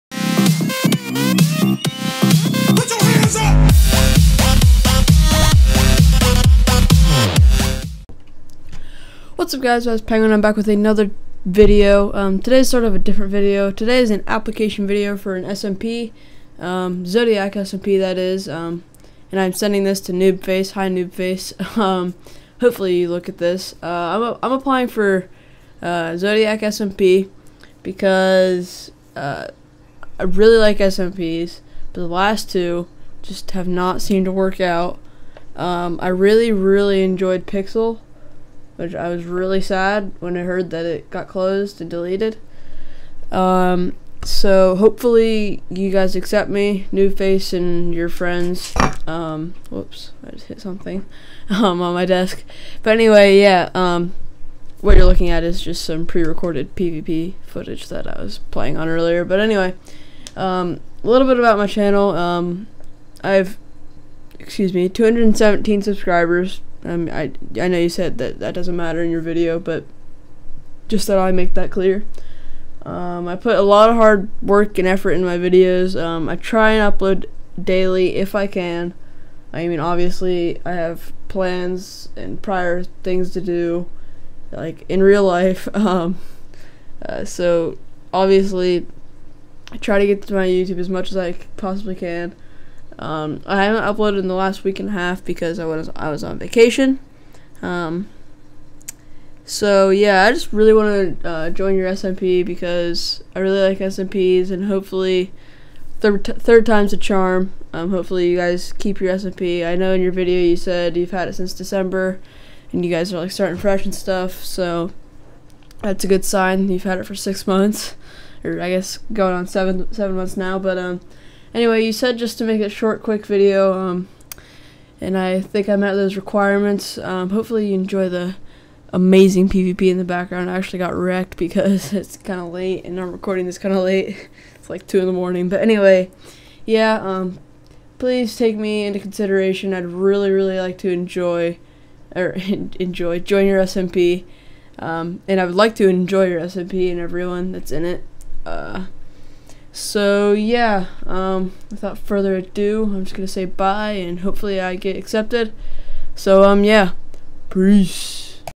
Zodiac SMP Application - Penguinverse
Hey guys i'm back with another video. Today I am applying for a new SMP that I will hopefully get accepted to, 3rd time is the charm right?